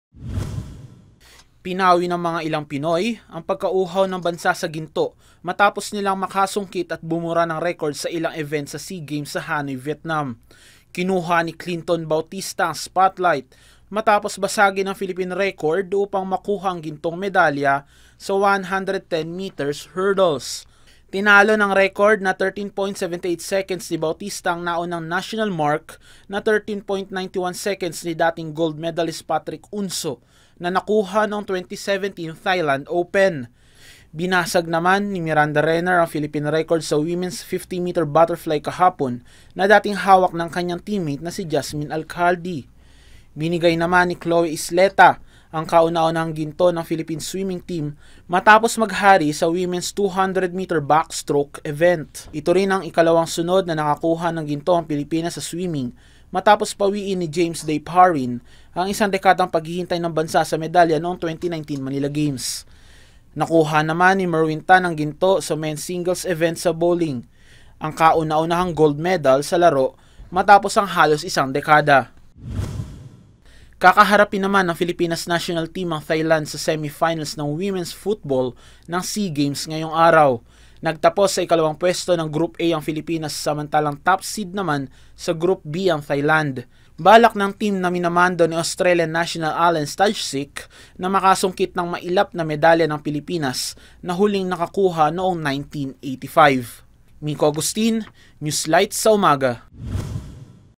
Pilipinas, patuloy na bumibida sa 31st SEA Games
PERFORMANCE LEVEL 🥇

Sa pagpapatuloy ng 31st Southeast Asian Games sa Hanoi, Vietnam, bumida ang ilang Pinoy matapos ang sunod-sunod na ginto sa iba’t ibang sporting event.

Samantala, kailangang talunin ng Filipinas National Team ang Thailand Women's Team ngayong araw sa semi-finals ng Women's Football upang mapalakas ang kanilang tyansa na makuha ang ginto sa SEA Games.